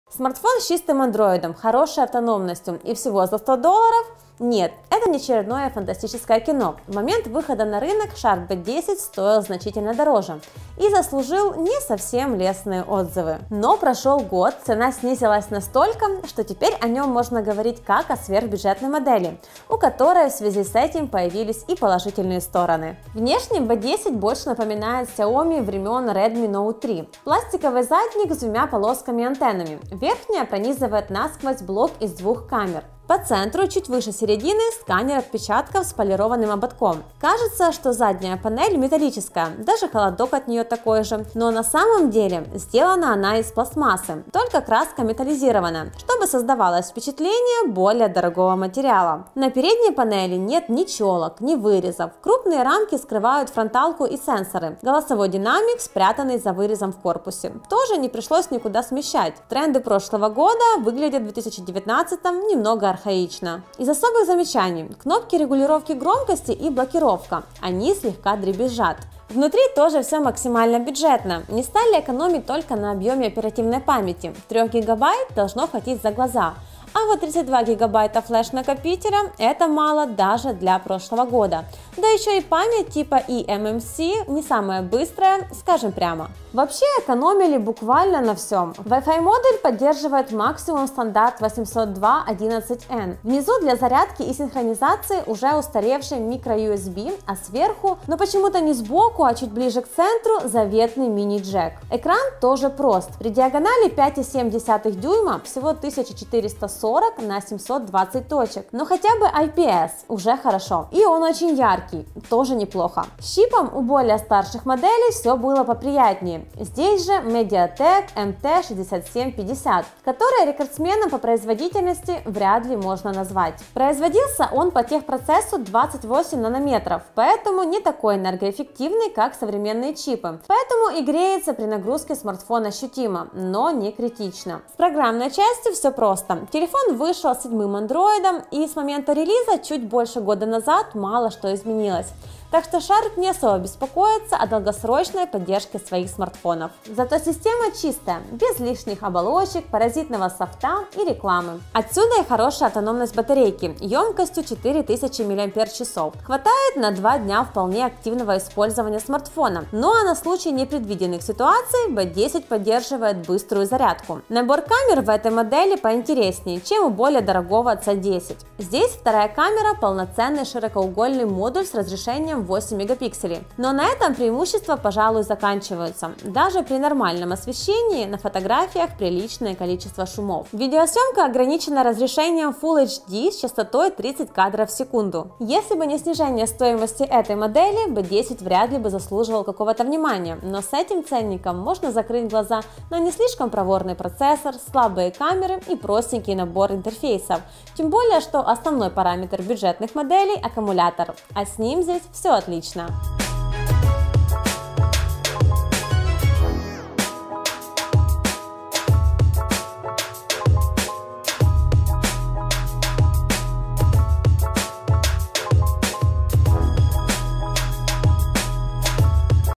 Sharp B10 - автономный бюджетник за $100
Недорогой, с чистой системой Android, долгоиграющий, несмотря на простенький чип - Sharp B10 стал интересней после того, как его цена серьезно снизилась.